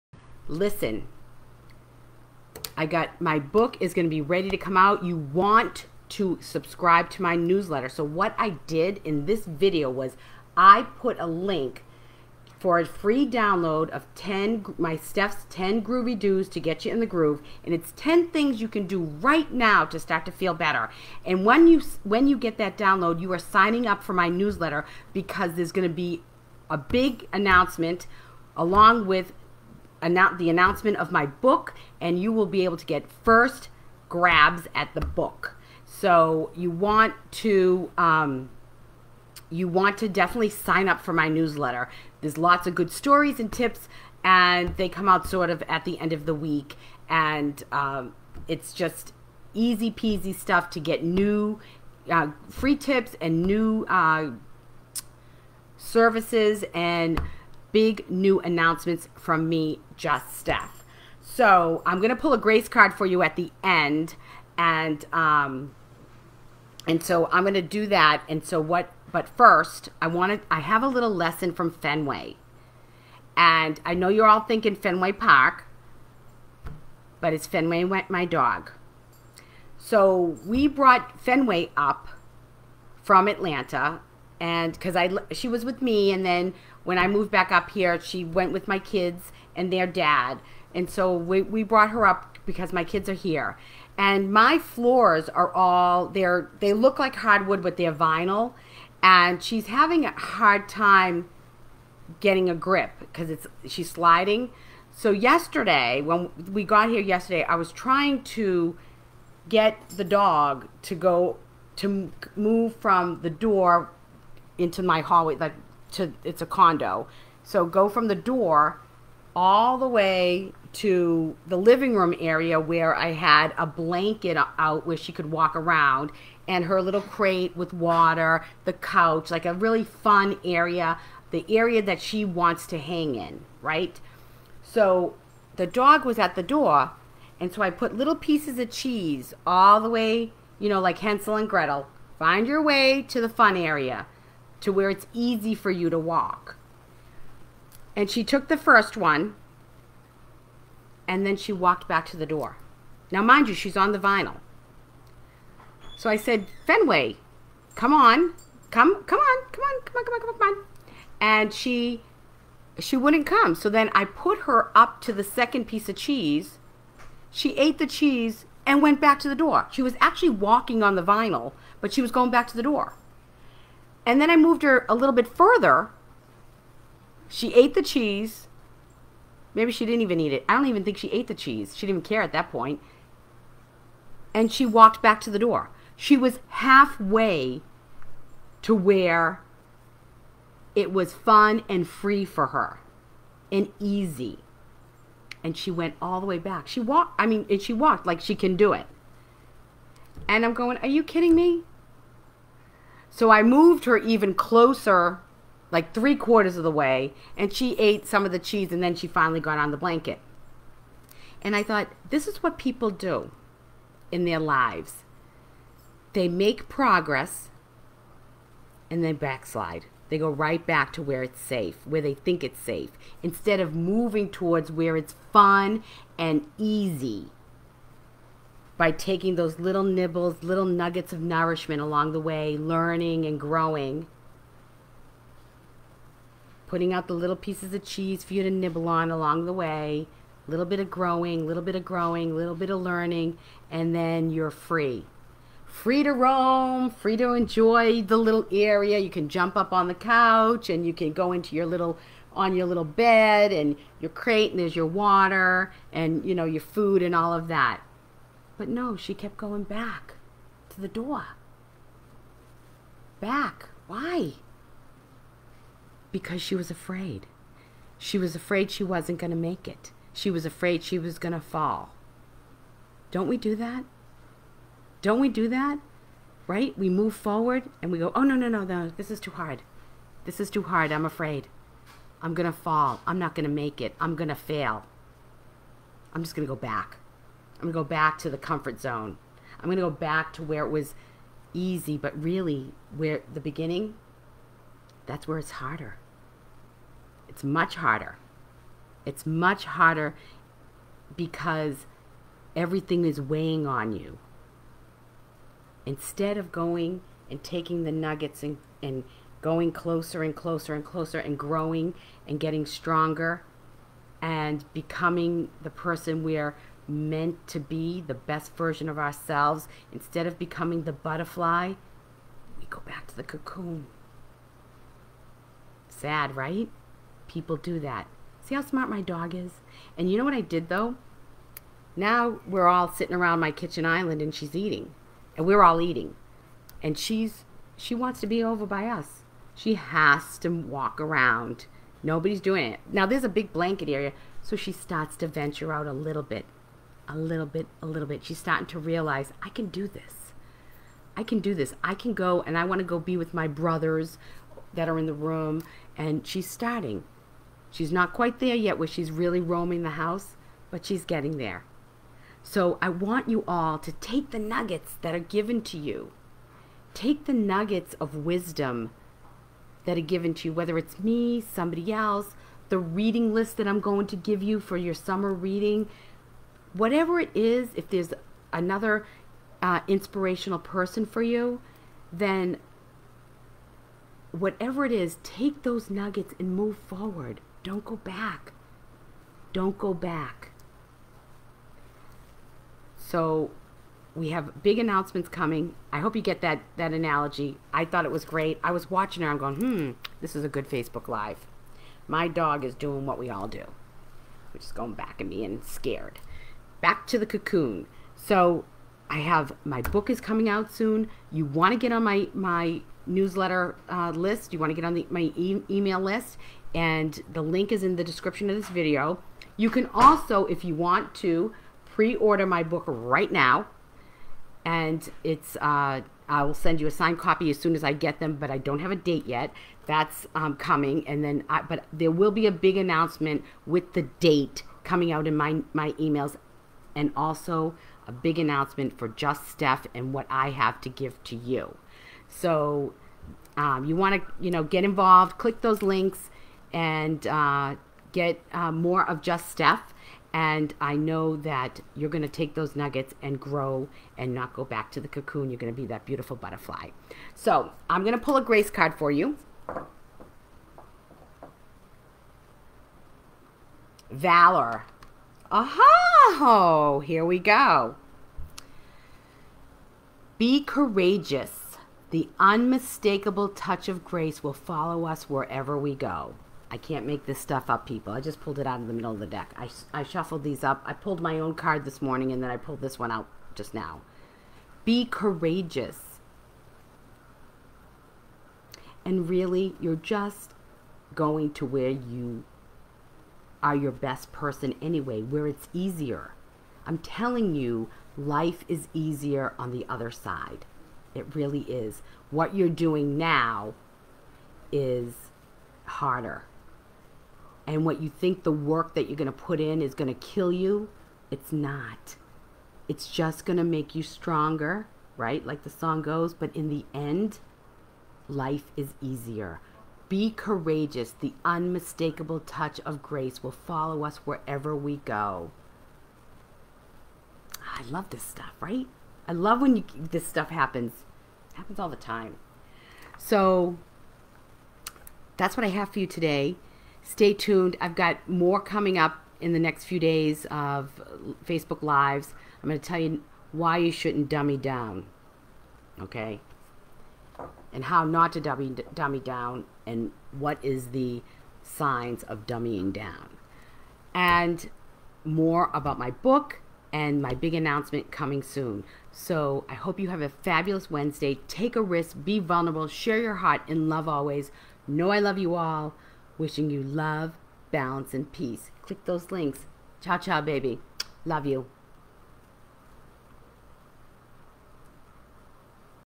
Lessons From Fenway (TM)
Trying to get my dog to walk across my vinyl floors, I get a glimpse of how we all behave when we are trying to move forward in life.